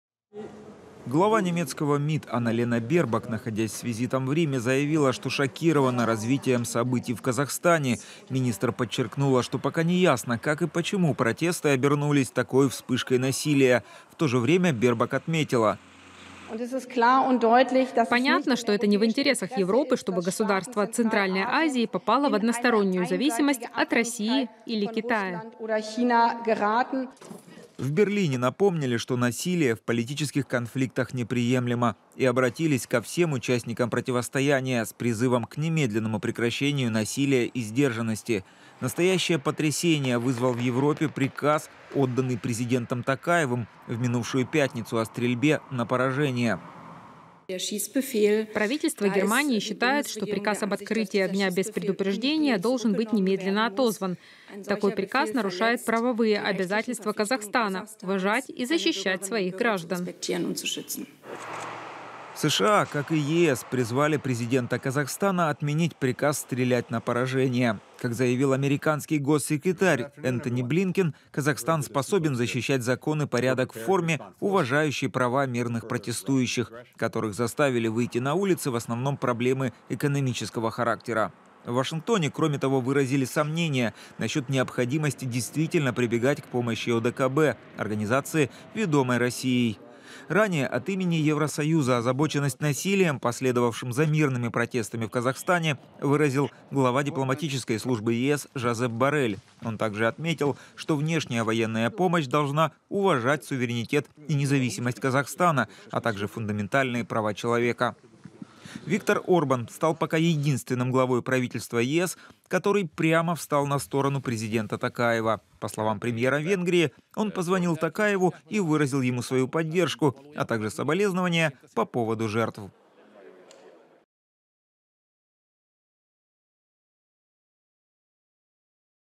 Берлин призывает Токаева отменить приказ о стрельбе на поражение
В Европе шокированы вспышкой насилия в Казахстане, в которую переросли мирные акции протеста из-за повышения цен на топливо. Отменить приказ о стрельбе на поражение, который отдал президент Токаев, призывают в США и ЕС.